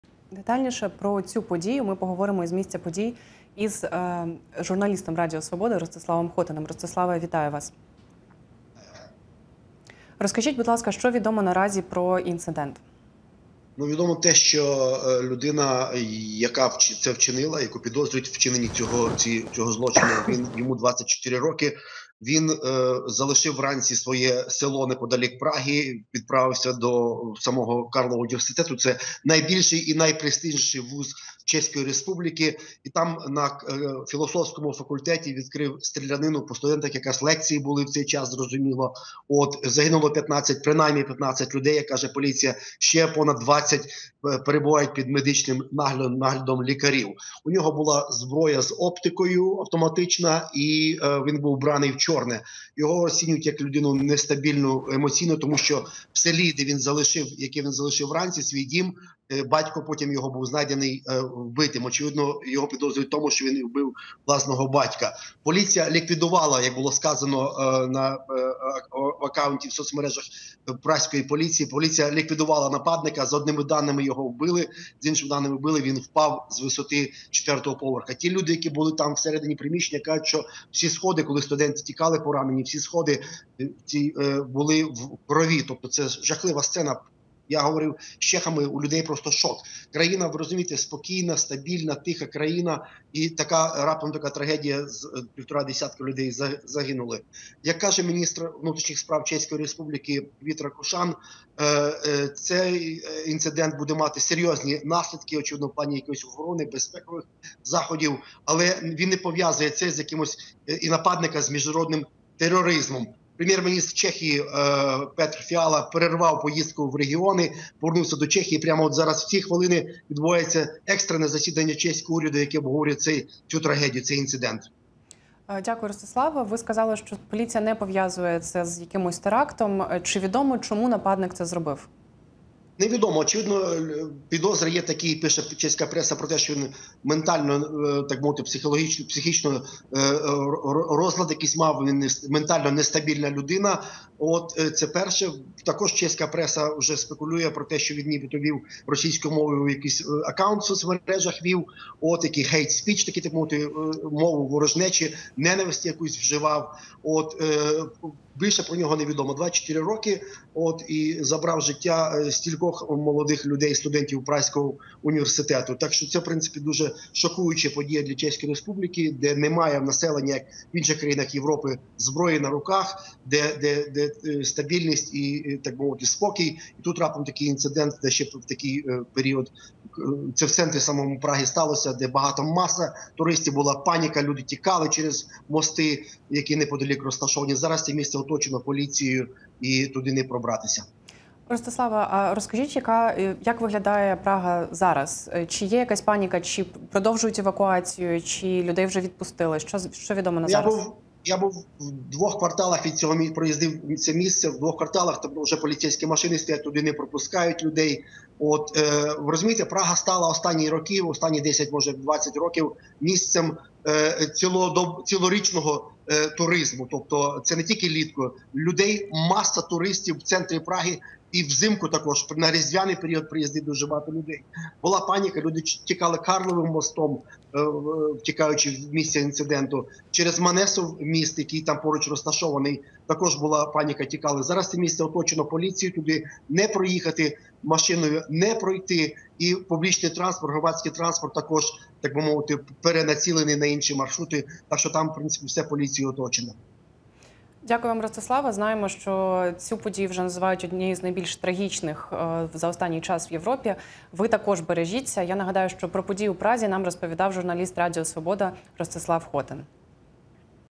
Що відомо про масову стрілянину у Празі?
Щонайменше 15 людей загинули і десятки отримали поранення внаслідок стрілянини у Карловому університеті в Празі. Нападник відкрив вогонь близько 3-тьої години дня за місцевим часом у будівлі вишу, розташованого на площі Яна Палаха у старій частині міста. Поліція стрільця ліквідувала. Чи ідентифікувала поліція нападника та чи відомі мотиви? Подробиці.

Слідкуйте за подіями у світі, США та Україні на вебсторінці та у соціальних мережах української служби Голосу Америки.